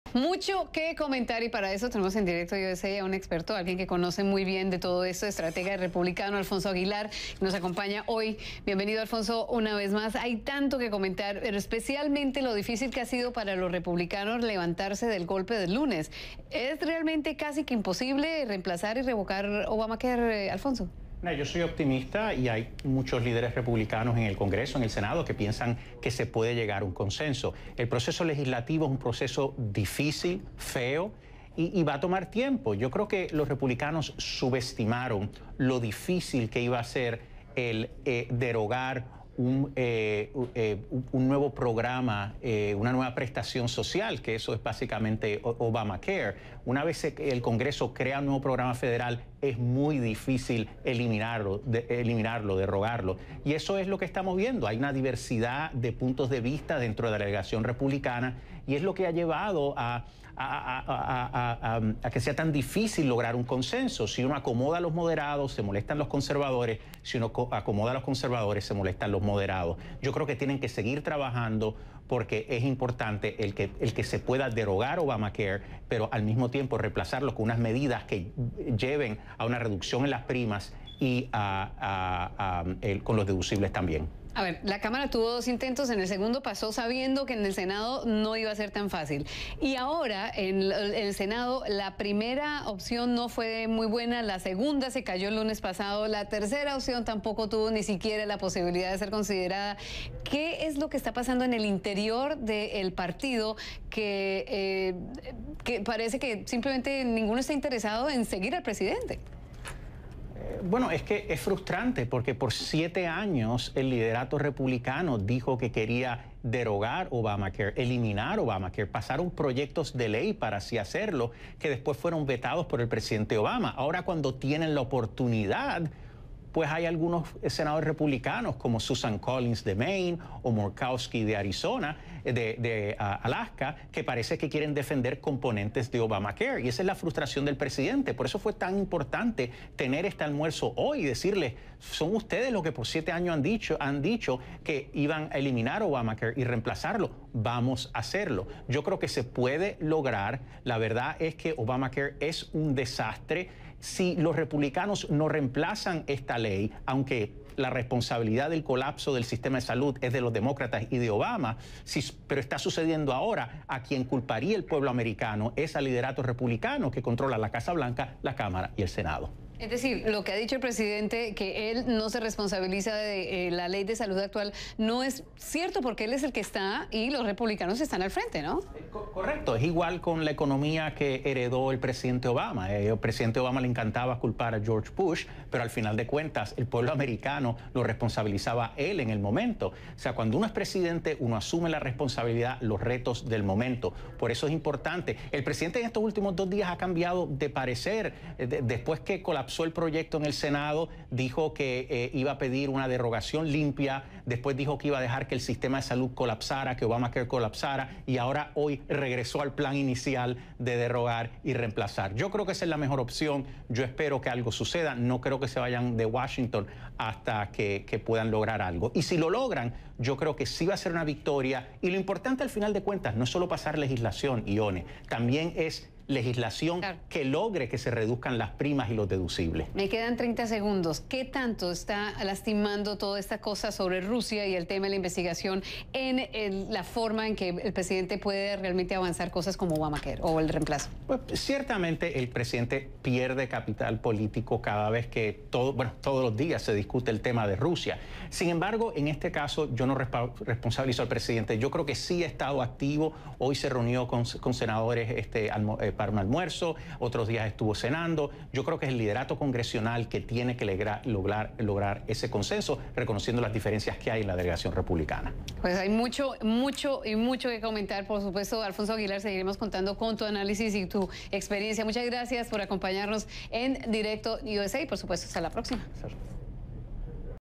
El futuro del remplazo de la ley de salud
El estratega republicano Alfonso Aguilar  explica el punto en el que se encuentra el esfuerzo de los repubicanos por cambiar la ley de salud actual conocida como Obamacare.